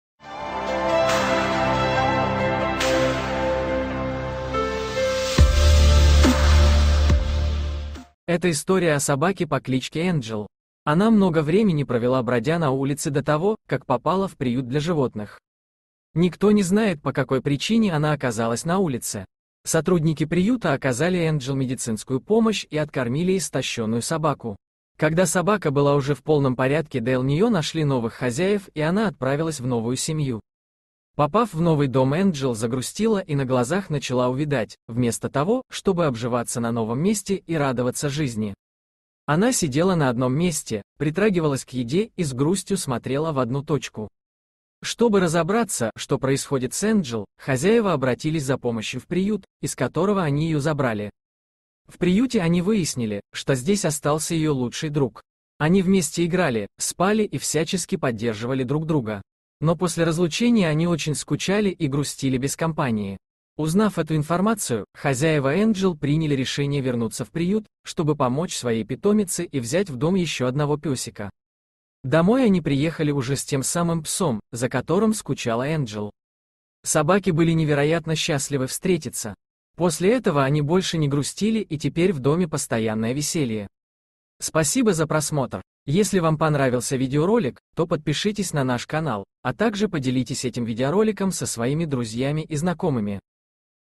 Собаку взяли из приюта и она начала увядать, но случилось невероятное
Эта история о собаке по кличке Энджел. Она много времени провела бродя на улице до того, как попала в приют для животных. Никто не знает по какой причине она оказалась на улице. Сотрудники приюта оказали Энджел медицинскую помощь и откормили истощенную собаку. Когда собака была уже в полном порядке дл нее нашли новых хозяев и она отправилась в новую семью.